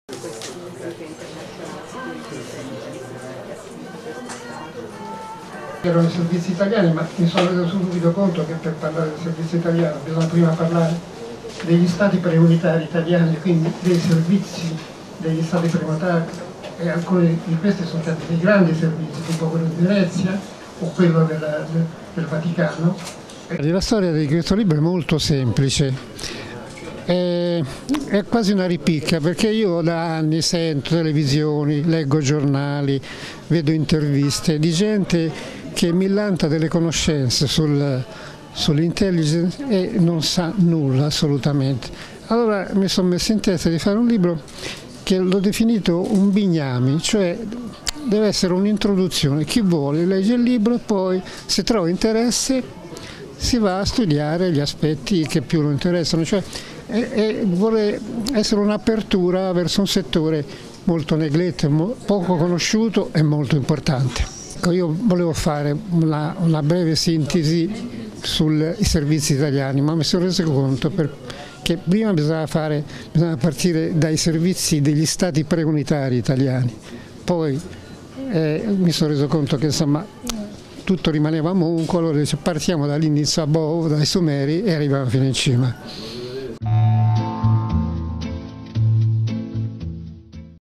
libro generale mori
L'intelligence itaina raccontata dall'interno. E' stato presentato, oggi alla Feltrinelli a Palermo il libro "Servizi segreti", scritto dal generale Mario Mori.